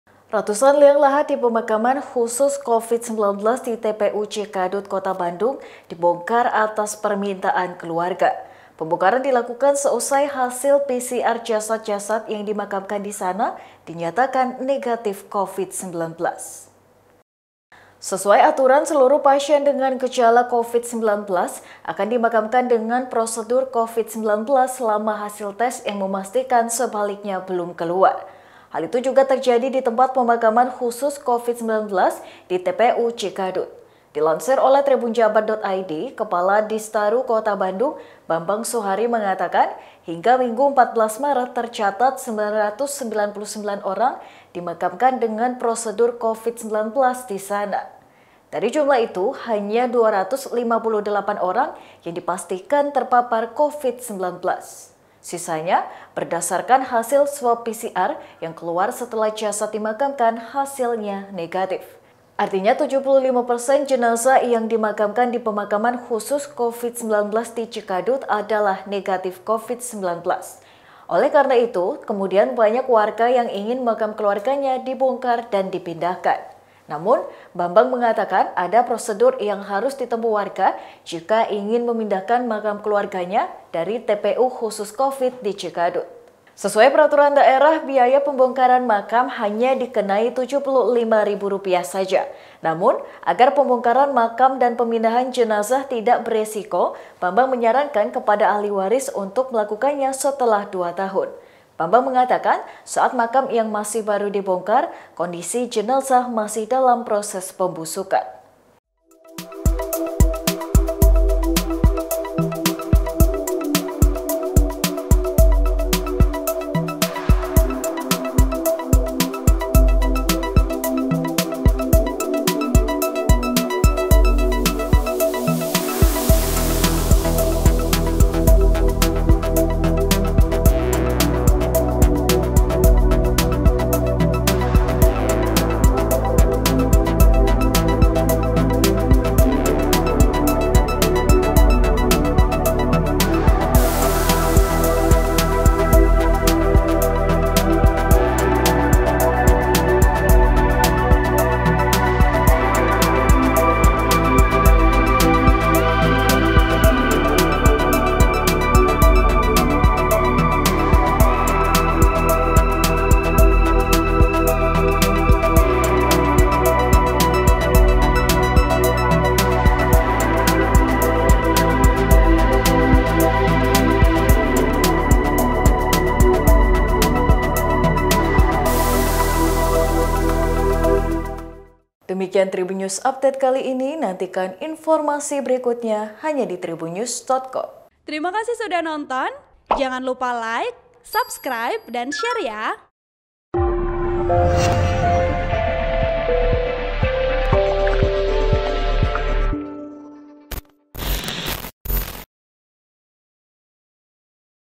Ternyata 75 Persen Jenazah di Pemakaman Khusus Covid-19 di Cikadut Negatif Covid-19
TRIBUN-VIDEO.COM - Ratusan liang lahat di pemakaman khusus Covid-19 di TPU Cikadut, Kota Bandung, dibongkar atas permintaan keluarga.

Pembongkaran dilakukan seusai hasil PCR jasad-jasah yang dimakamkan di sana dinyatakan negatif Covid-19.

Sesuai aturan, seluruh pasien dengan gejala Covid-19 akan dimakamkan dengan prosedur Covid-19 selama hasil tes yang memastikan sebaliknya belum keluar.

Hal itu juga terjadi di tempat pemakaman khusus Covid-19 di TPU Cikadut.

Dilansir oleh TribunJabar.id, Kepala Distaru Kota Bandung, Bambang Suhari mengatakan, hingga Minggu (14/3) tercatat 999 orang dimakamkan dengan prosedur Covid-19 di sana.

Dari jumlah itu, hanya 258 orang yang dipastikan terpapar Covid-19.

Sisanya, berdasarkan hasil swab PCR yang keluar setelah jasad dimakamkan, hasilnya negatif.

Artinya, 75 persen jenazah yang dimakamkan di Pemakaman Khusus Covid-19 di Cikadut adalah negatif.

Itu sebabnya, kemudian banyak warga yang ingin makam keluarganya dibongkar dan dipindahkan.

"Hingga Minggu, 14 Maret, sudah 153 makam yang dibongkar," ujar Bambang kepada Tribun Jabar saat dihubungi melakui telepon, semalam.

Namun, Bambang mengatakan, ada prosedur yang harus ditempuh warga jika ingin memindahkan makam keluarganya dari TPU khusus Covid di Cikadut.

"Pertama, harus ada surat keterangan dari rumah sakit yang menyatakan bahwa almarhum negatif Covid-19. Kedua, harus ada surat penerimaan dari pengelola pemakaman baru yang dituju. Ketiga, pembongkaran, pengangkutan, dan pemakaman kembali, harus memakai standar Covid," kata Bambang.

Sesuai peraturan daerah, ujar Bambang, biaya pembongkaran makam hanya Rp 75 ribu.

Namun, agar pembongkaran makam dan pemindahan jenazah tidak berisiko, Bambang menyarankan kepada ahli waris untuk melakukannya setelah dua tahun.

Bambang mengatakan, saat makam yang masih baru dibongkar, kondisi jenazah masih dalam proses pembusukan.

(Tribun-video.com/TribunJabar.id)

Artikel ini telah tayang di tribunjabar.id dengan judul WADUH, 75 Persen Jenazah di Pemakaman Khusus Covid-19 di Cikadut Ternyata Negatif Covid-19,
Penulis: Mega Nugraha
Editor: Ravianto